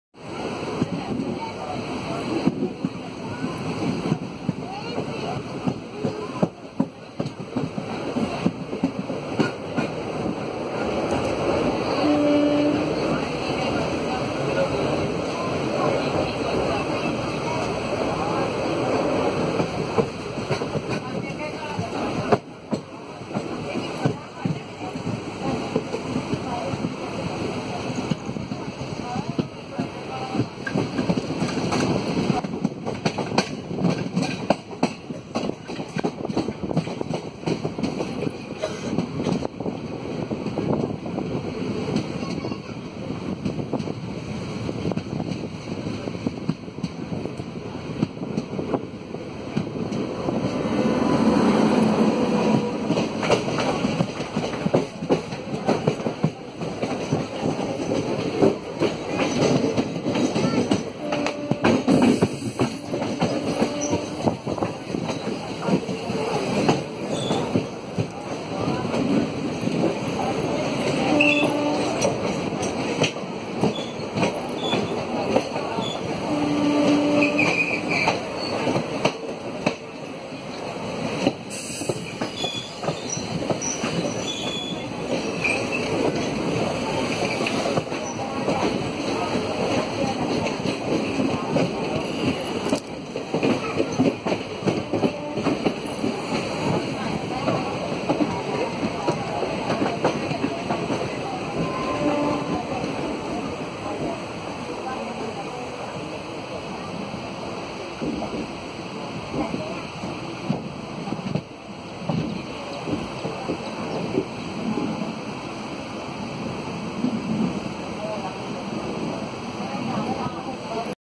Falling Sun & Rising WAP7 Defeats Darkness @ Drizzling Dusk
💥💥💥  HAPPY DIWALI  💥💥💥  May This Festival Fill Our Mind With Peace And Tranquility And Let Our Journey Be Towards The Never Ending Light And Prosperity.

16792 Palakkad Junction - Punalur  Palaruvi Express crossing Royapuram (RPM) WAP7 30342 hauled Alappuzha - Chennai Central Superfast Express under overcast weather near Manjali Rivulet (near Angamaly railway station) towards dusk.